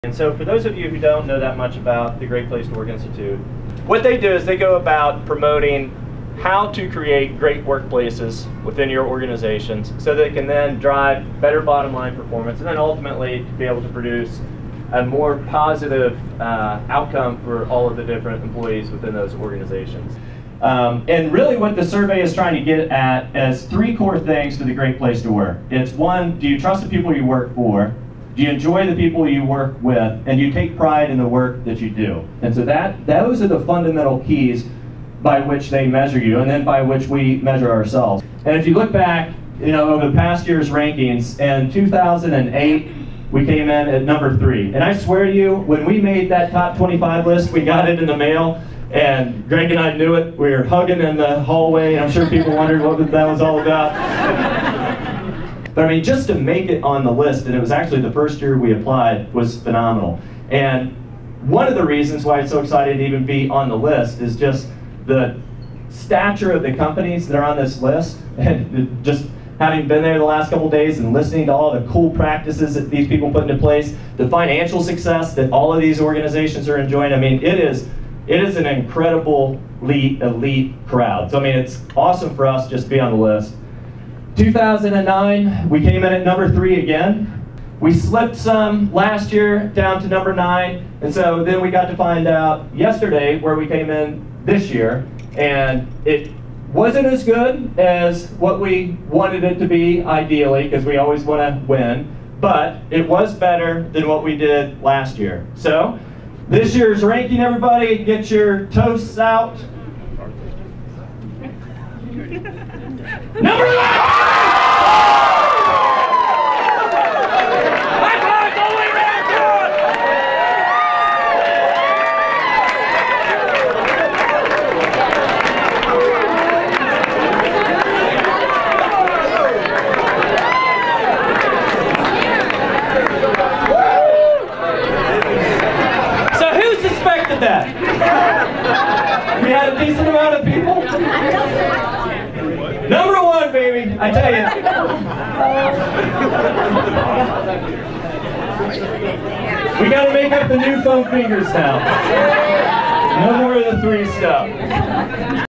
Snagajob Great Place to Work Announcement
CEO Shawn Boyer announces where Snagajob ranks in 2011's Best Small Business to Work for in America